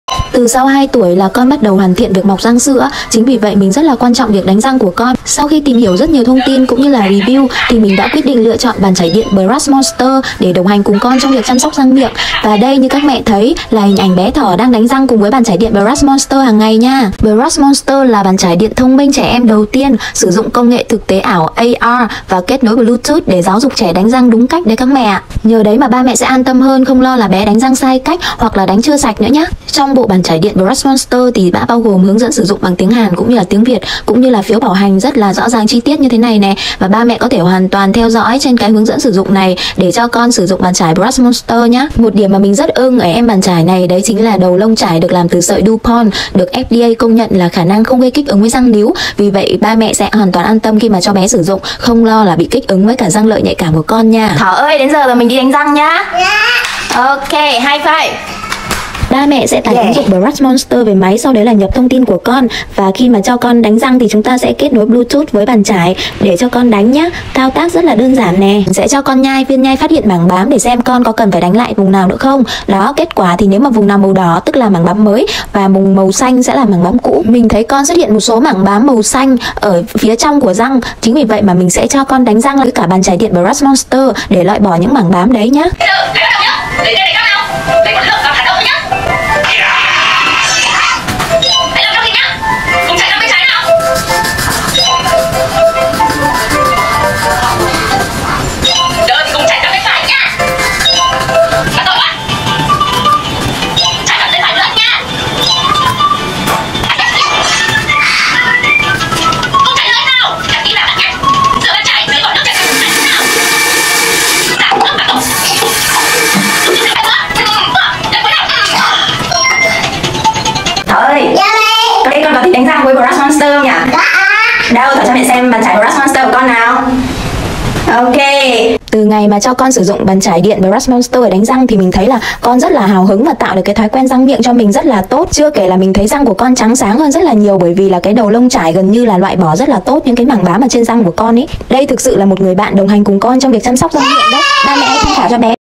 Bàn chải điện Brush Monster - lựa chọn của mẹ khi chăm sóc răng miệng cho bé
Làm thế nào để giúp bé chăm sóc răng miệng đúng cách?

Chị Trang chia sẻ:
"Từ sau 2 tuổi, răng sữa của con đã bước vào giai đoạn hoàn thiện nên ngoài việc quan tâm tới bữa ăn, giấc ngủ của con thì mình luôn dành sự quan tâm đặc biệt cho việc chăm sóc răng miệng của bé".

"Ba mẹ có thể chưa biết, nhưng theo tạp chí chăm sóc răng miệng BMC Oral Health thì cứ 2 trẻ nhỏ thì có 1 bé không bao giờ chải mặt trong của răng và cứ 4 trẻ nhỏ đánh răng hàng ngày thì có 1 bé bị sâu răng. Điều này làm mình thực sự rất lo lắng. Con gặp vấn đề về răng miệng sẽ ảnh hưởng không chỉ về thẩm mỹ mà còn về sức khỏe của con nữa. Đánh răng không thôi là chưa đủ mà còn phải đánh đúng cách, có đánh giá để ba mẹ biết được tình trạng răng miệng của con".

"Sau thời gian tìm hiểu thì mình lựa chọn bàn chải điện Brush Monster để đồng hành cùng con trong việc chăm sóc răng miệng. Không giống như những bàn chải điện thông thường khác, Brush Monster được ví như nha sĩ AI cá nhân mang lại phản hồi về quá trình chải răng của bé".

"Có một người bạn, một nha sĩ AI như Brush Monster chăm sóc răng miệng cùng con mình thực sự rất an tâm. Từ hồi đó, bé Thỏ hào hứng với việc đánh răng lắm, và mình thấy răng của con trắng sáng hơn rất nhiều so với việc dùng bàn chải thông thường".

Ba mẹ quan tâm có thể tham khảo chi tiết sản phẩm tại: